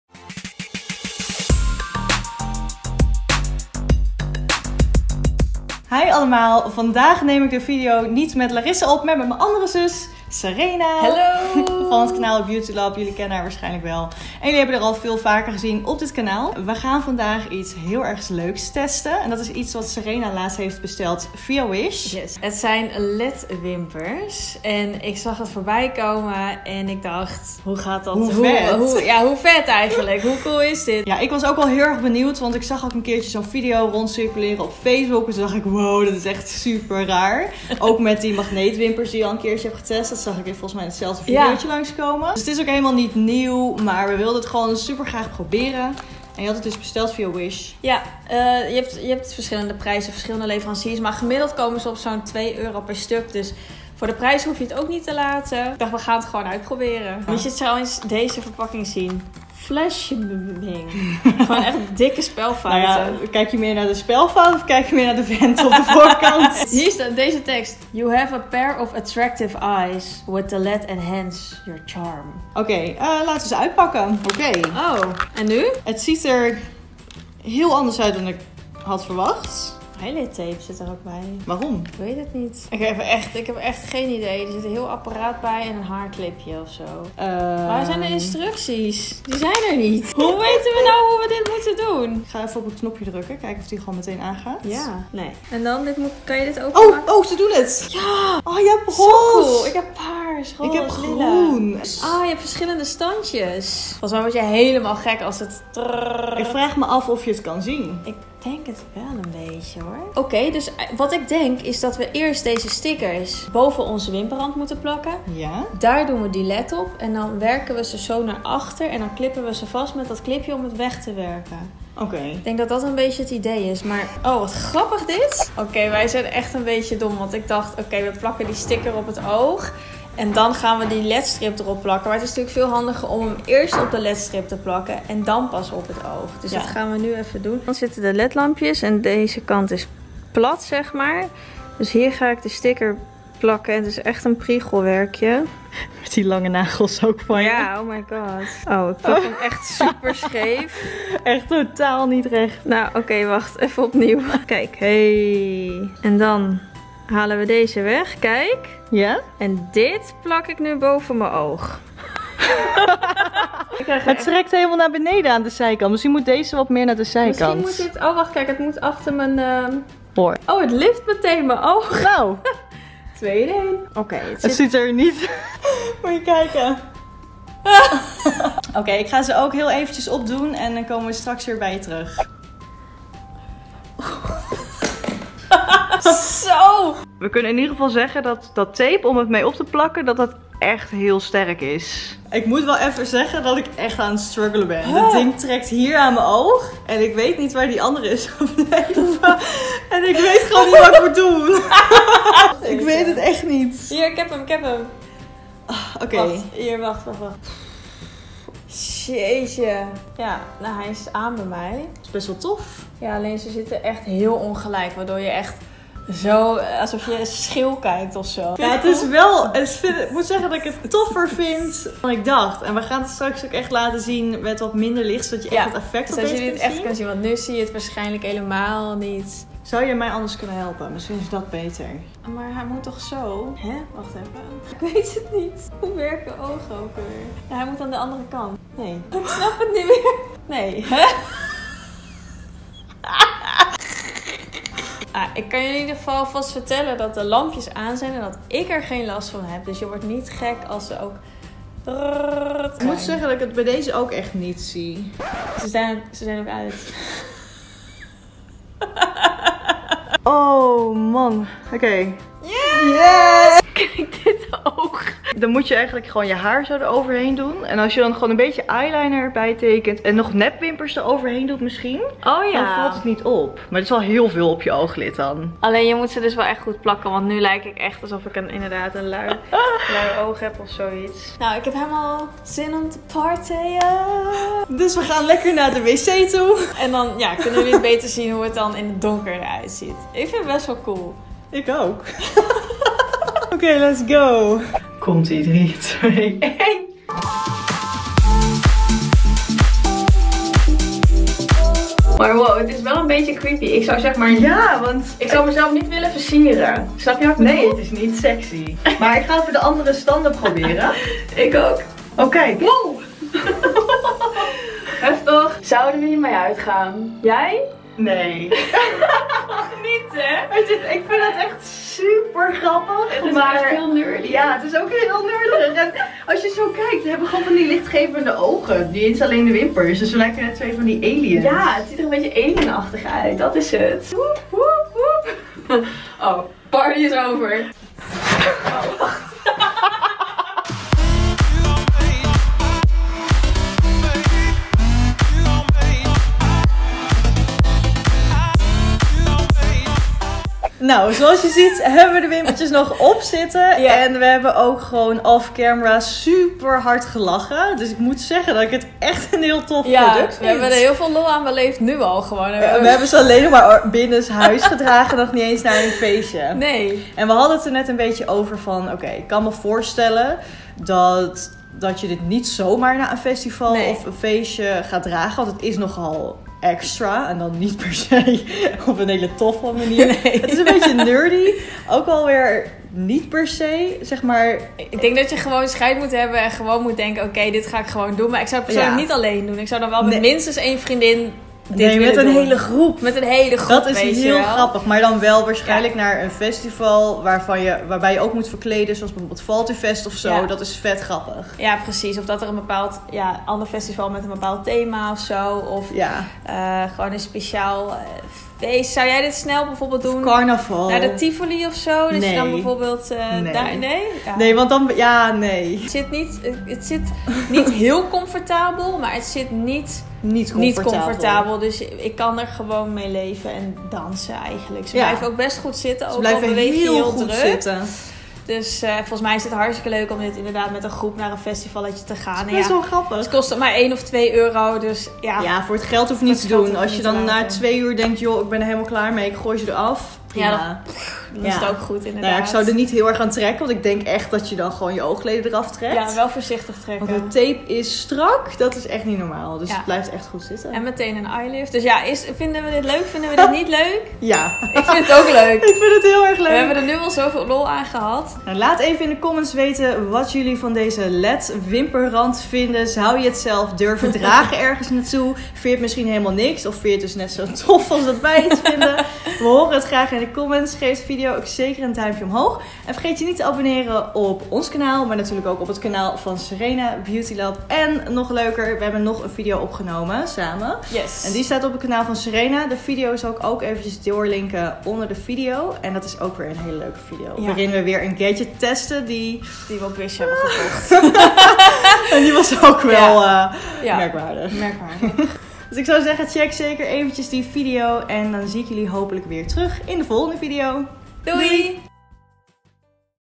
WISH LED LASHES TESTEN MET SERENA (BEAUTYLAB)
We testen led strips voor op de ogen van Wish.

P.s. Fijne Valentijnsdag!

▸ Serena's top:
▸ Tara's top: Primark

P R E V I O U S

▸ Snapchat: Endless_Tara / Endless_Larissa